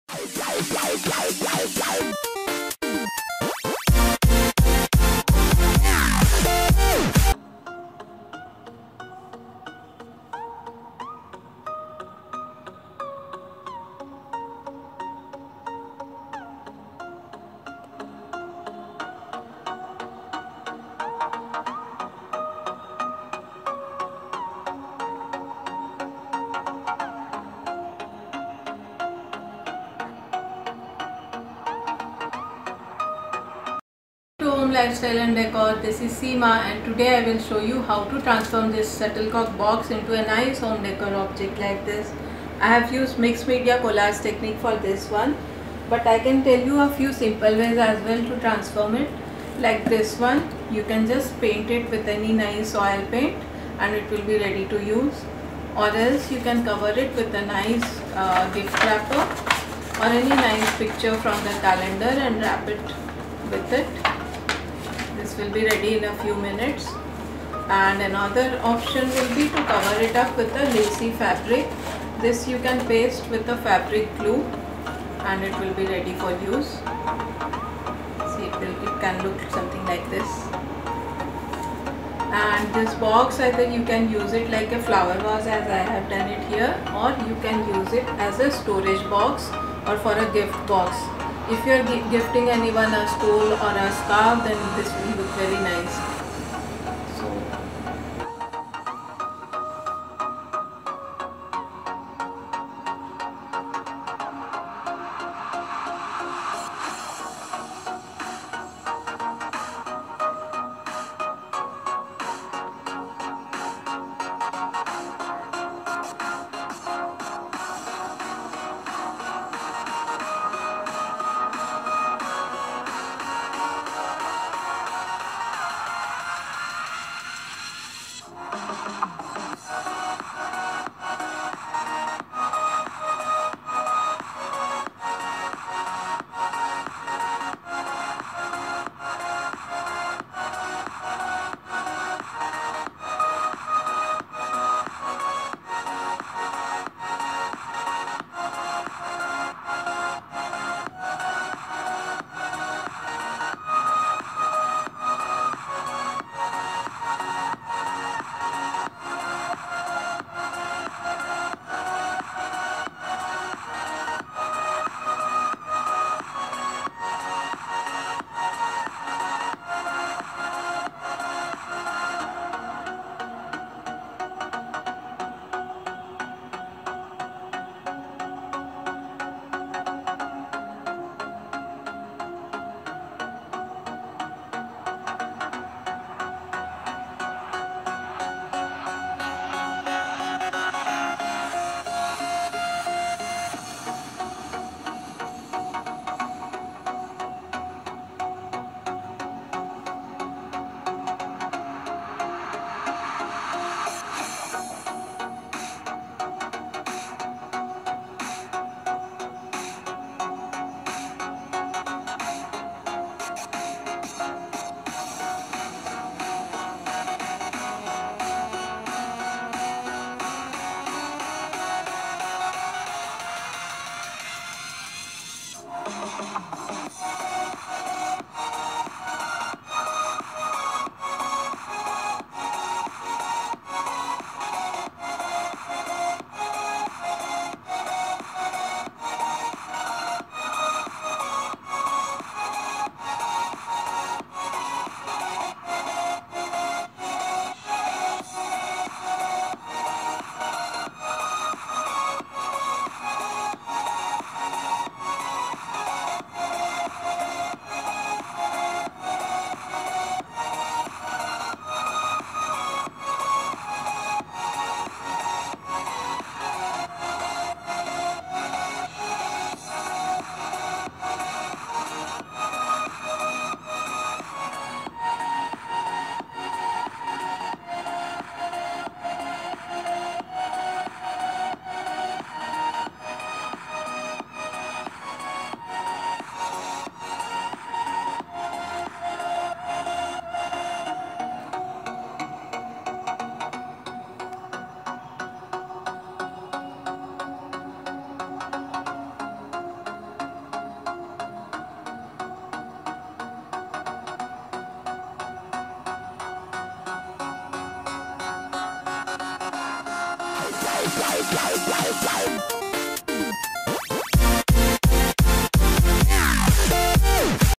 Best Out of Waste || Reusing Badminton Shuttlecocks Box
Best Out of Waste || Different ways of reusing Empty Badminton Shuttlecocks Box - DIY

If you like the ideas, Do connect to me for more such Home Décor and Organizing tips at